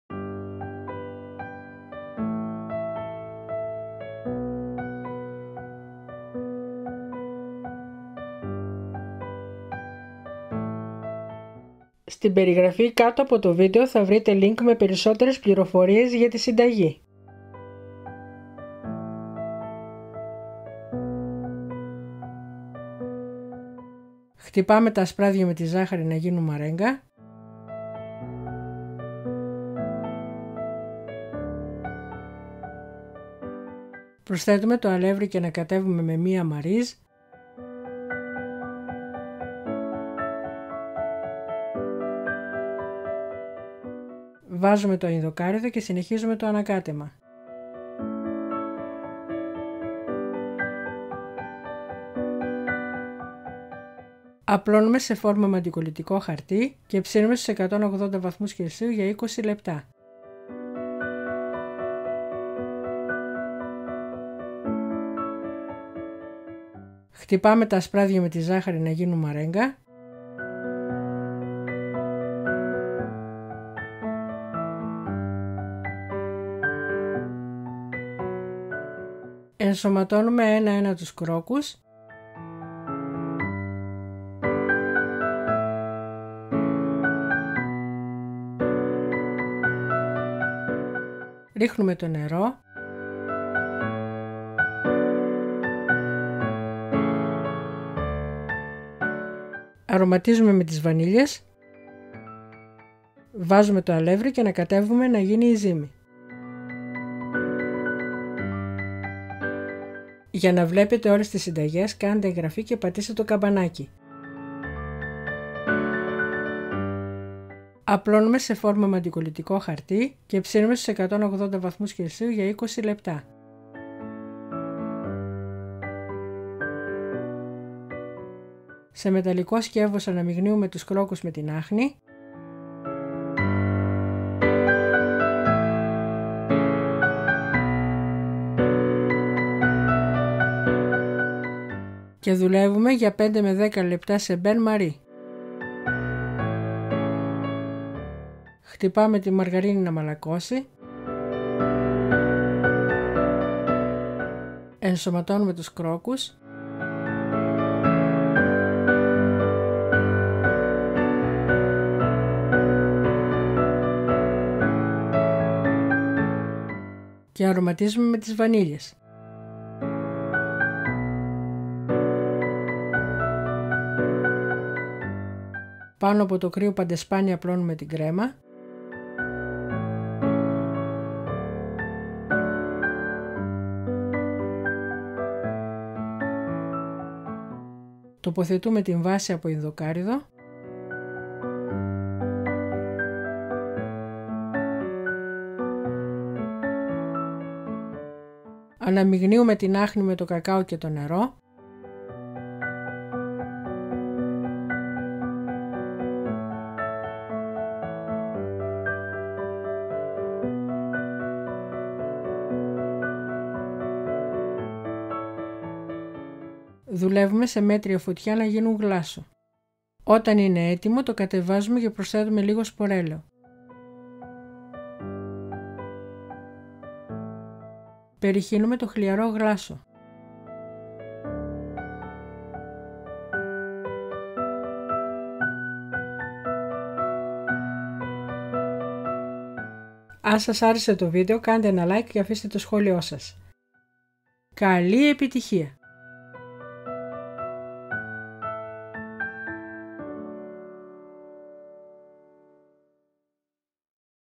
Αφράτα παστάκια με ινδοκάρυδο | Foodaholics
Αφράτα παστάκια με ινδοκάρυδο | Afrata pastakia me indokarudo | Foodaholics

Συστατικά:

Παντεσπάνι καρύδα
 6 ασπράδια
 100 γρ ζάχαρη
 2 κουταλιές φαρίνα
 100 γρ ινδοκάρυδο
 Παντεσπάνι
 6 ασπράδια
 6 κρόκους
 75 γρ ζάχαρη
 7 κουταλιές φαρίνα
 2 βανίλιες
 300 γρ μαργαρίνη
 6 κρόκους
 100 γρ άχνη
 3 βανίλιες
 Γλάσο
 3 κουταλιές κακάο
 150 γρ άχνη
 2 κουταλιές νερό
 1 κουταλιά σπορέλαιο